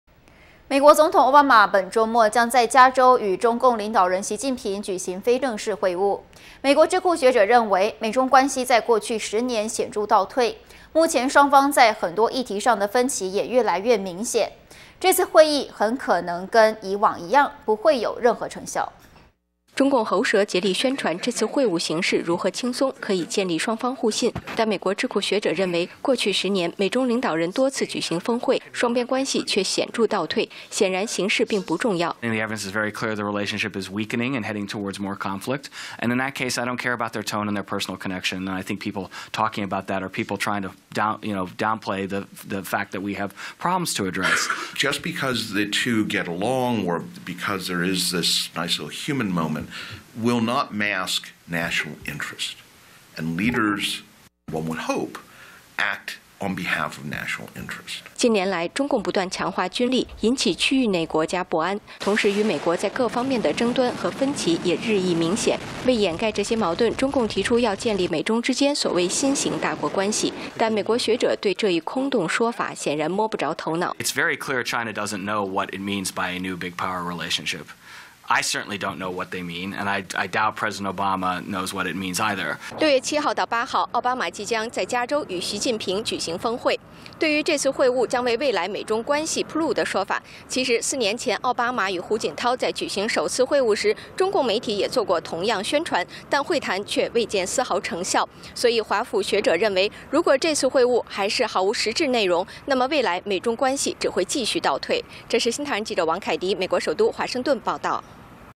美中關係趨倒退 歐習會難有成效【習近平_中國真相最新新聞】
【中國真相新聞】(China Breaking News) 美國總統歐巴馬本週末將在加州與中共領導人習近平舉行非正式會晤。美國智庫學者認為，美中關係在過去十年顯著倒退，目前雙方在諸多議題上的分歧也日益明顯。這次會晤很可能如以往一樣，不會有任何成效。【新唐人亞太台2013年6月6日訊】

中共喉舌竭力宣傳這次會晤形式如何輕鬆，可以建立雙方互信。但美國智庫學者認為，過去十年，美中領導人多次舉行峰會，雙邊關係卻顯著倒退，顯然形式並不重要。

美國傳統基金會高級研究員 史劍道：「證據很清楚，美中關係是在惡化，並趨向更多爭端。在此情況下，他們對話的氣氛和個人接觸並不重要。講這話的人是想淡化我們面臨的問題。」

美國傳統基金會高級研究員 成斌：「只是他們兩人處得不錯、有個人接觸，並不能掩蓋國家利益。而我們期待領導人將代表國家利益行事。」

近年來，中共不斷強化軍力，引起區域內國家的不安，同時與美國在各方面的爭端和分歧日益明顯。為掩蓋這些矛盾，中共提出要建立中美之間的所謂「新型大國關係」，但美國學者對這一空洞說法顯然摸不著頭腦。

美國傳統基金會高級研究員 史劍道：「很清楚，中共不知道什麼是所謂新型大國關係。我也不知道，恐怕歐巴馬總統也不知道。」

6月7日到8日，歐巴馬即將與習近平在加州舉行峰會。這也是習近平正式接班後與歐巴馬的首次會晤。對於所謂「峰會將為美中未來關係鋪路」的說法，4年前歐巴馬與胡錦濤首次會面時，中共也曾同樣宣傳，但會晤卻絲毫未見成效。所以學者認為，這次會議如果毫無實質內容，雙邊關係只會繼續倒退。